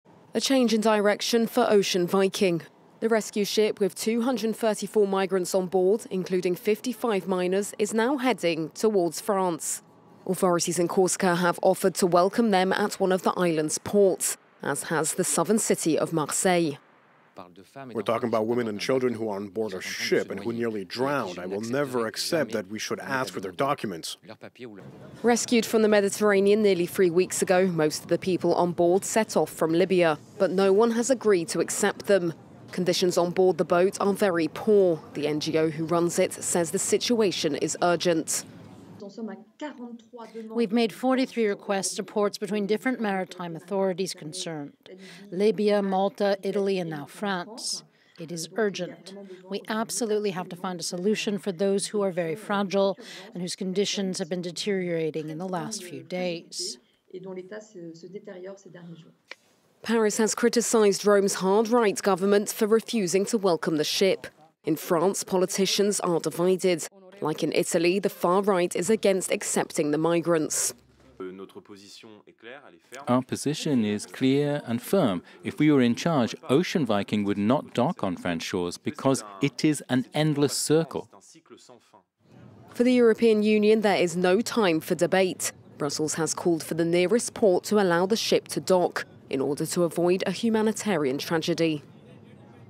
Migrant ship turned away by Italy heads towards France • FRANCE 24 English
French authorities have hit out at Italy over its refusal to allow a rescue vessel with 234 migrants onboard to dock. Paris called the move unacceptable. The Ocean Viking operated by SOS Mediterranee sailed away from Sicily towards France after failing to secure permission from Italian authorities to dock since late-October. FRANCE 24's Selina Sykes and Noémie Roche explain.